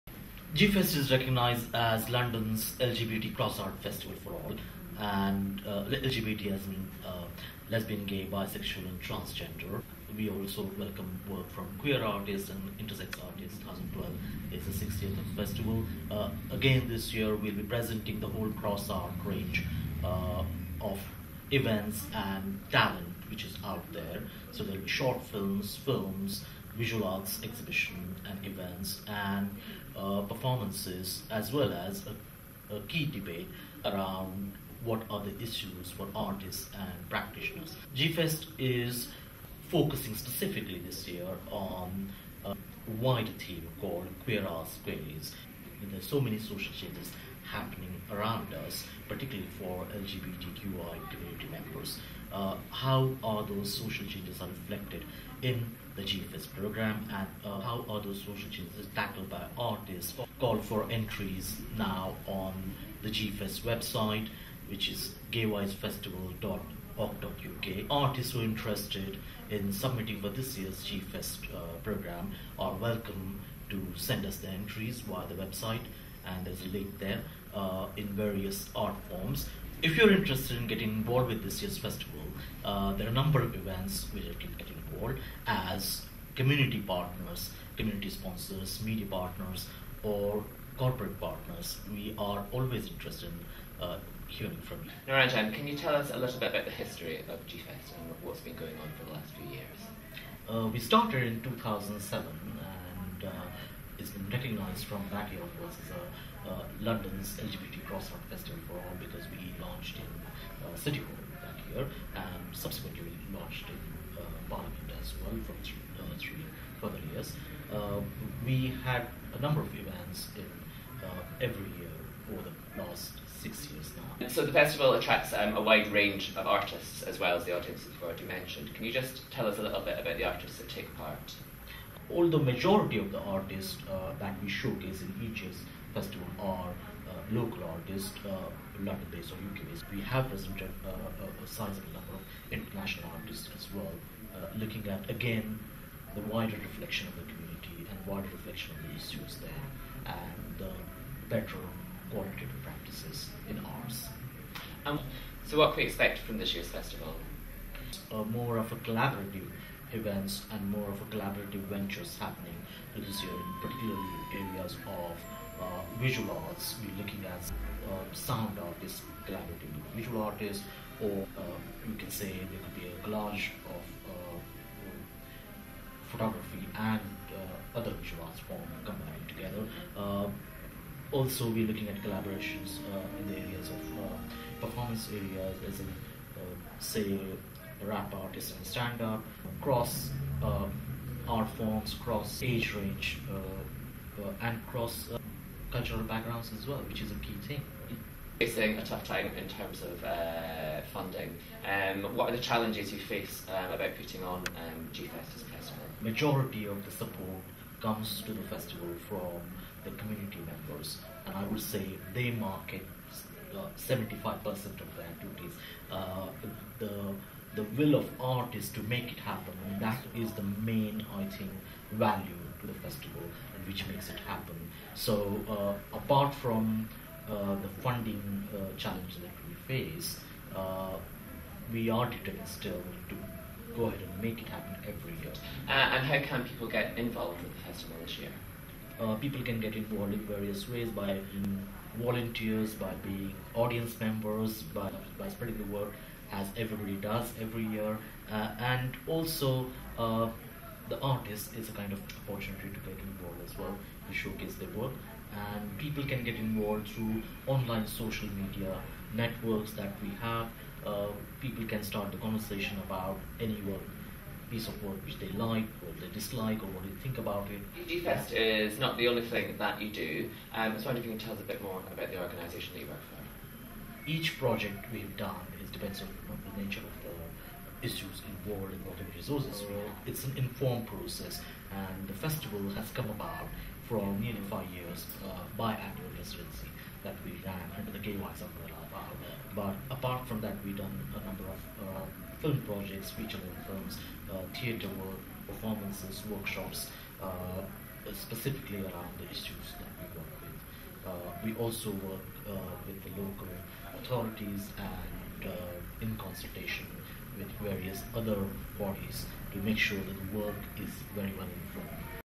GFEST 2012 Call for Entries
GFEST - Gaywise FESTival, London's LGBTQI cross - art festival for all - in London - announced 'Call for 2012 Entries'